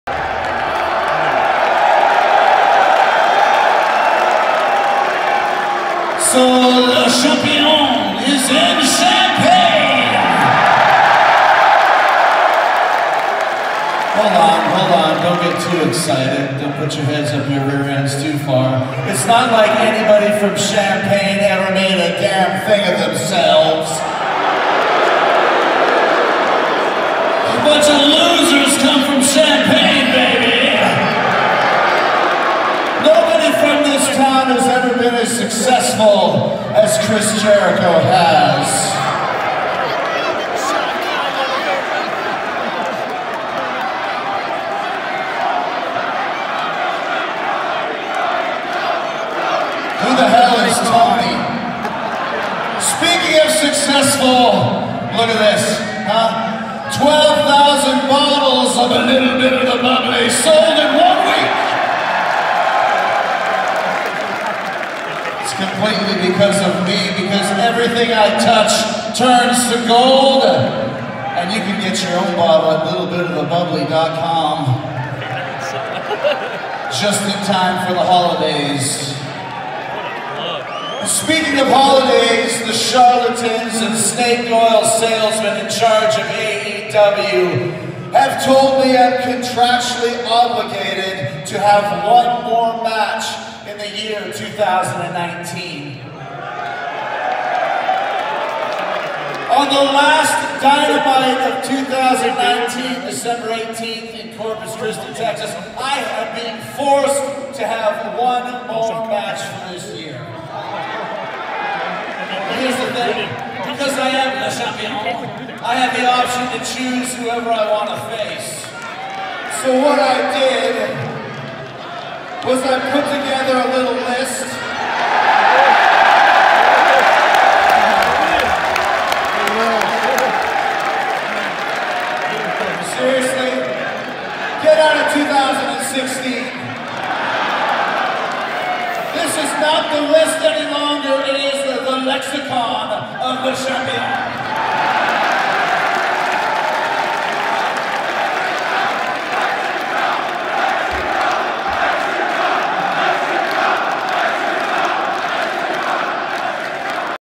Chris Jericho @ SFC in Champaign
Jericho interview on AEW Dynamite on 12-4-19